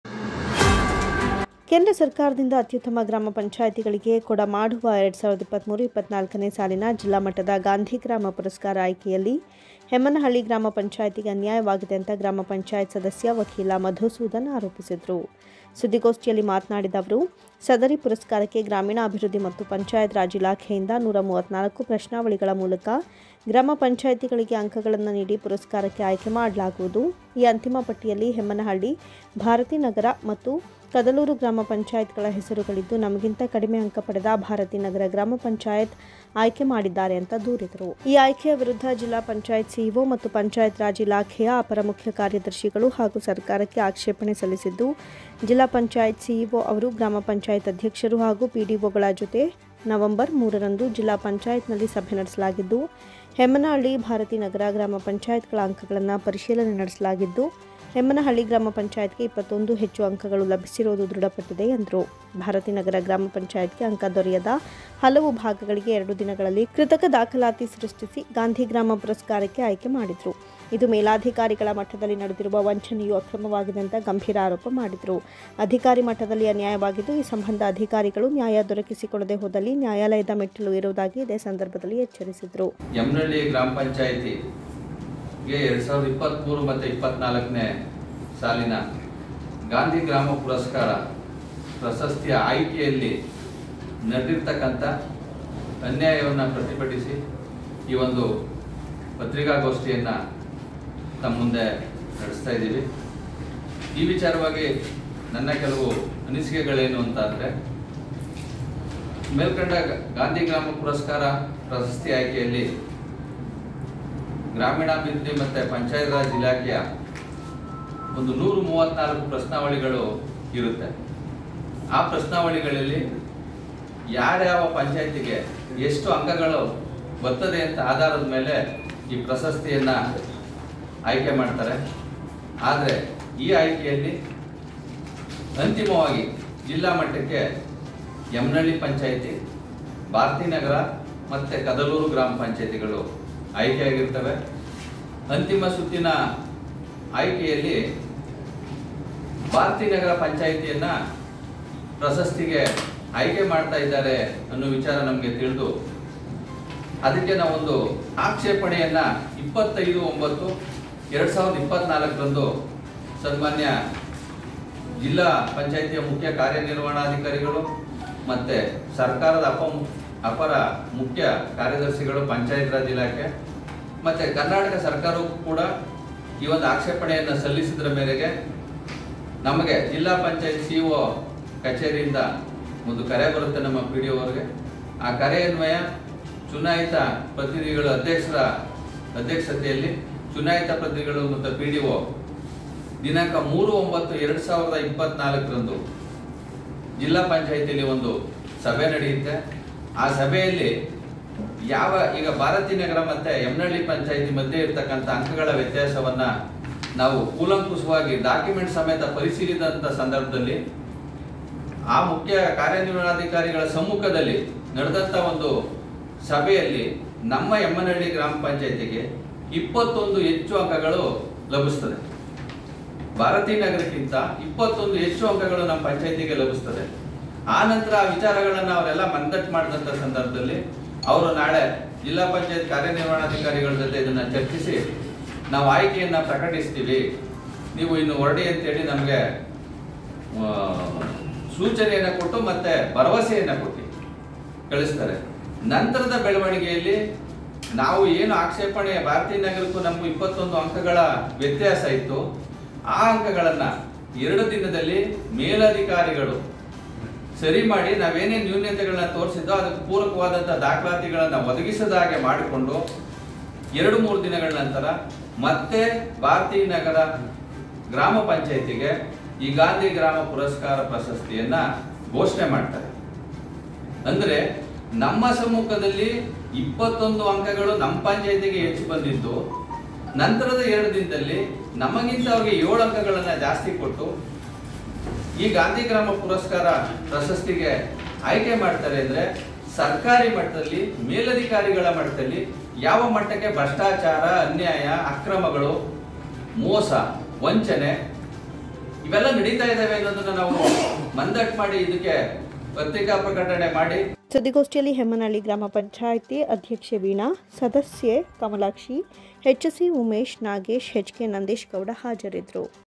ಜಿಲ್ಲಾ ಮಟ್ಟದ ಗಾಂಧಿ ಗ್ರಾಮ ಪುರಸ್ಕಾರ ಆಯ್ಕೆಯಲ್ಲಿ ಅಕ್ರಮ । ಹೆಮ್ಮನಹಳ್ಳಿ ಗ್ರಾ.ಪಂ.ಅಧ್ಯಕ್ಷೆ , ಸದಸ್ಯರ ಆರೋಪ
ಮಂಡ್ಯ : ಜಿಲ್ಲಾ ಮಟ್ಟದ ಗಾಂಧಿ ಗ್ರಾಮ ಪುರಸ್ಕಾರ ಆಯ್ಕೆಯಲ್ಲಿ ಅಕ್ರಮ । ಹೆಮ್ಮನಹಳ್ಳಿ ಗ್ರಾ.ಪಂ.ಅಧ್ಯಕ್ಷೆ , ಸದಸ್ಯರ ಆರೋಪ